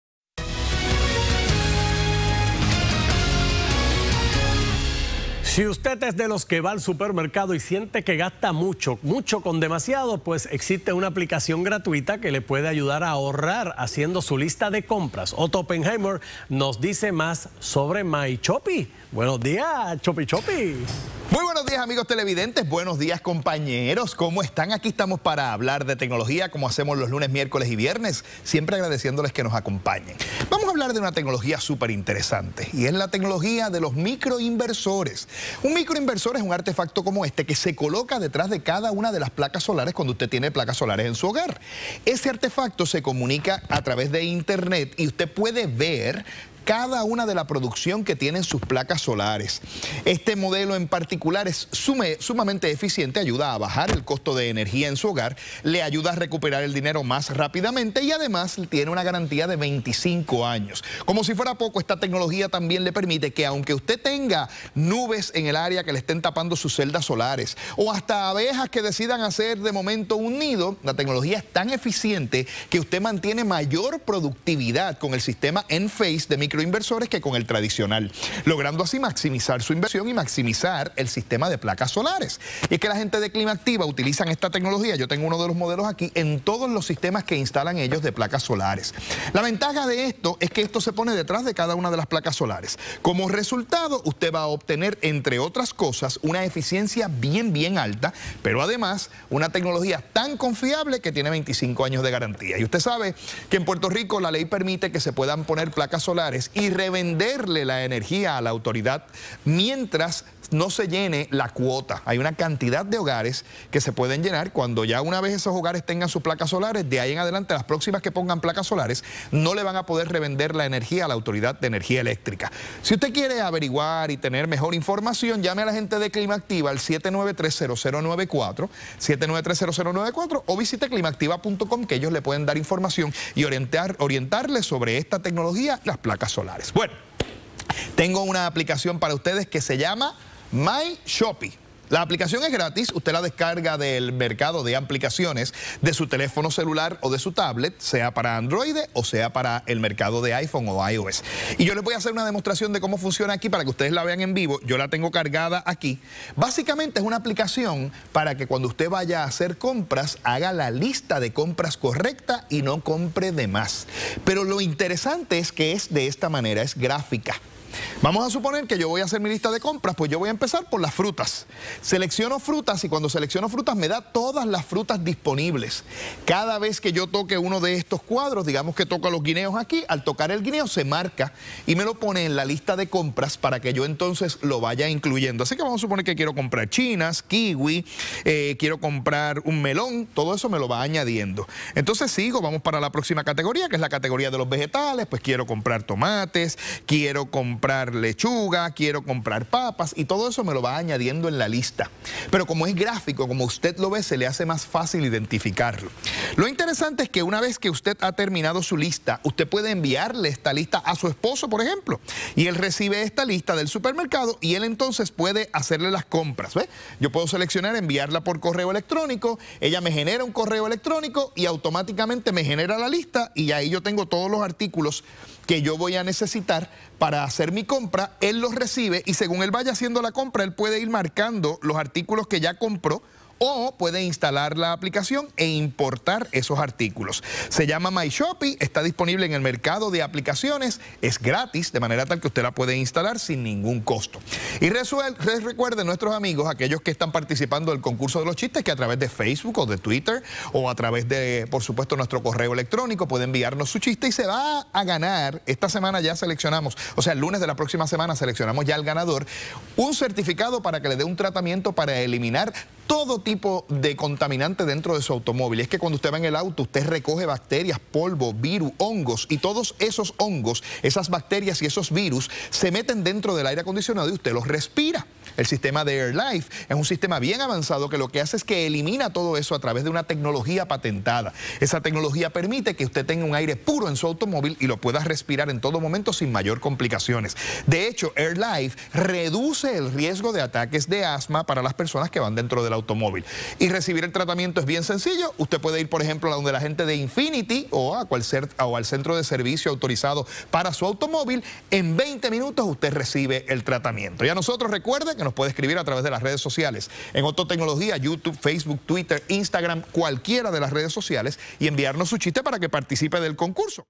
Otto Oppenheimer. Tecnoavances.
Hoy presentamos una tecnología que usada con placas solares maximiza la producción de energía, de Climactiva.
Un app que le permite hacer compras con imágenes, la lista de compras se puede enviar por correo electrónico a otras personas.